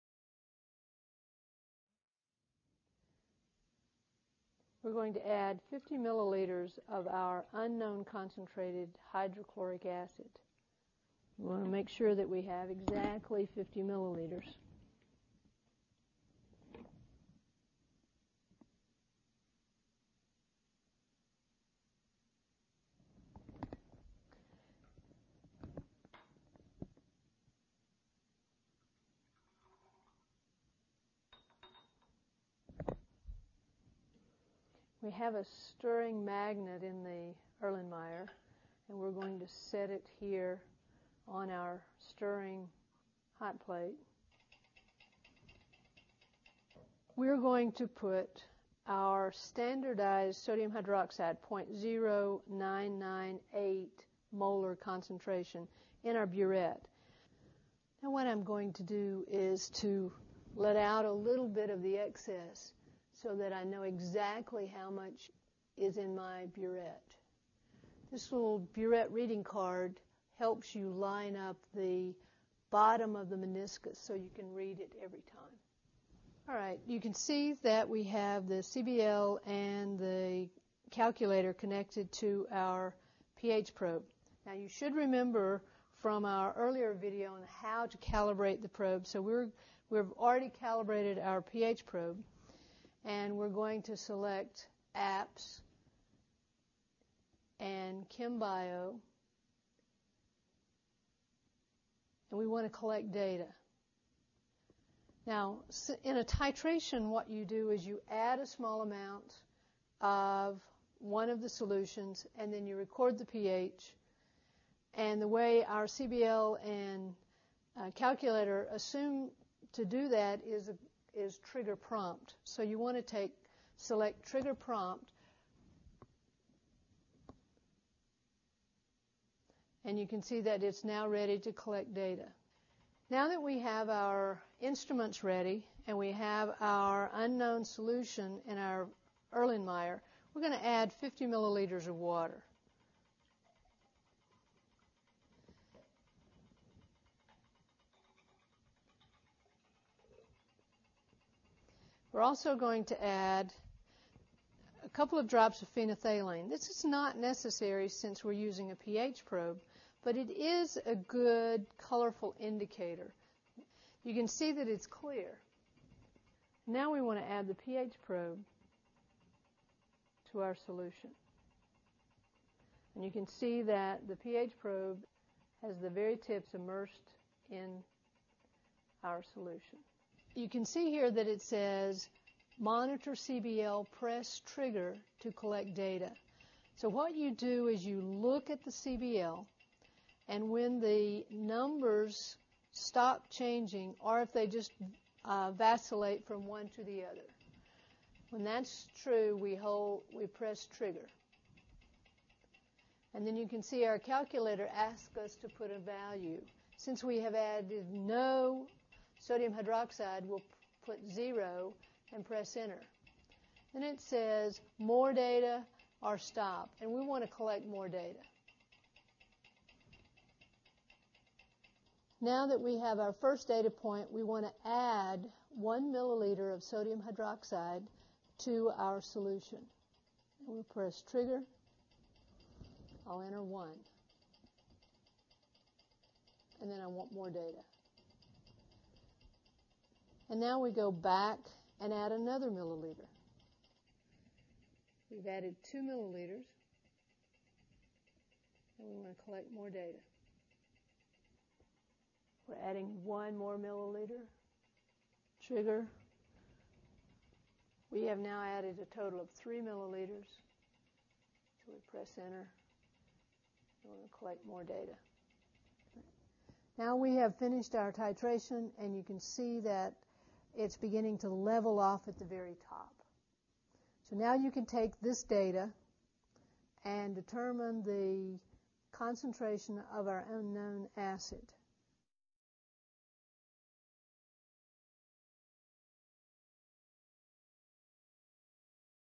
HCl Titration Lab
This video shows titration of a solution of HCl using a pH probe and CBL.